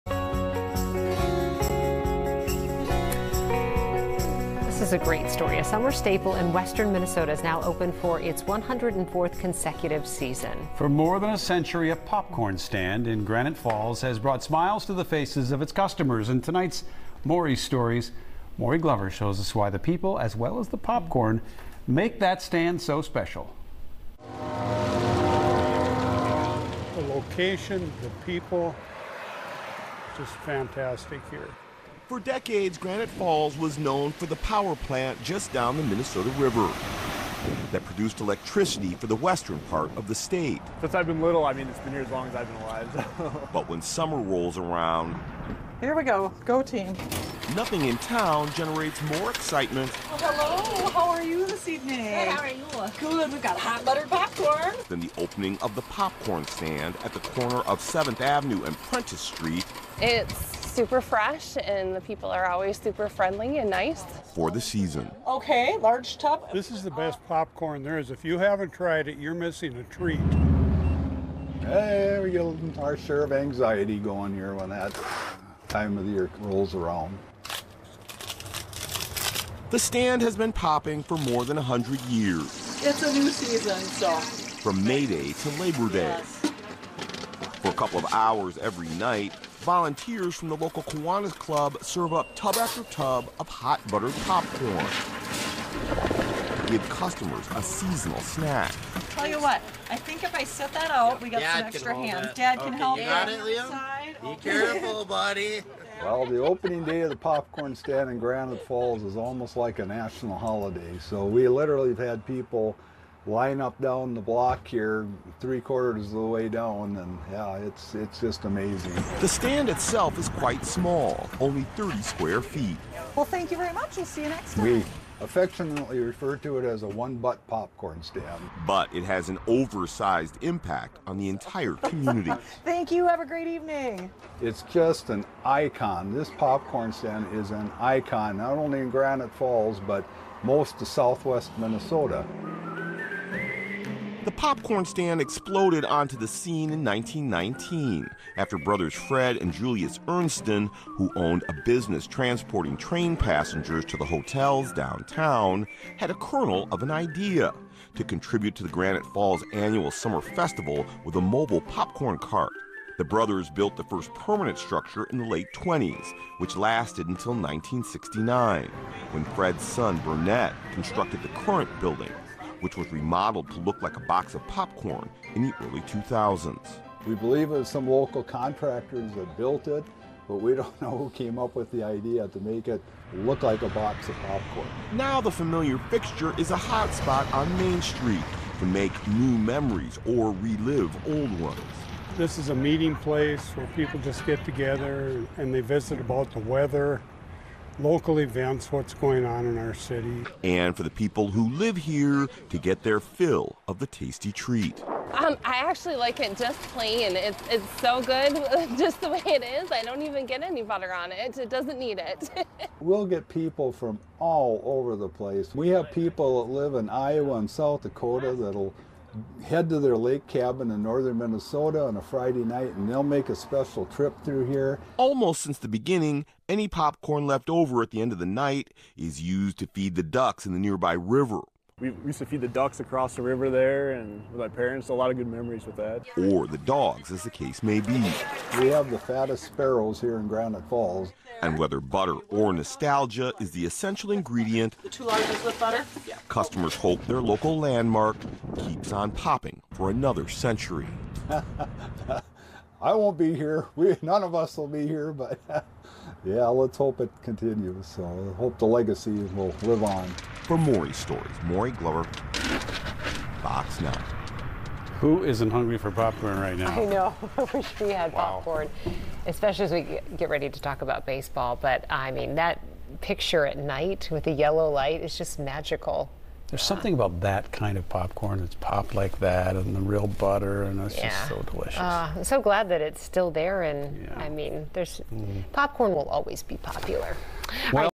Minnesota's century-old popcorn stand marks 104 years in Granite Falls
A summer staple in western Minnesota is now open for its 104th consecutive season.

For more than a century, a popcorn stand in Granite Falls has brought smiles to the lives of its customers.